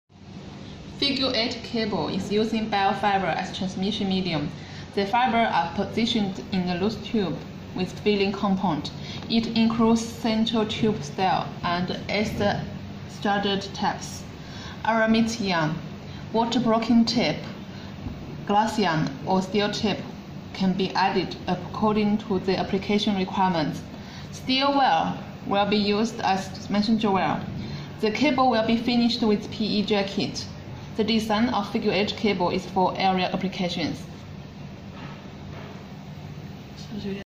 Figure 8 Fiber Optical Cable from RUXFiber in China
In the GYTC8S/GYTC8A cable, single-mode/multimode fibers are positioned in the loose tubes, while the loose tubes strand together around metallic central strength member into a compact and circular cable core, and the water-blocking materials are distributed into interstices of it. After a PSP/APL is applied around the cable core, this part of cable accompanied with the stranded wires as the supporting part are completed with a PE sheath to be a figure-8 structure.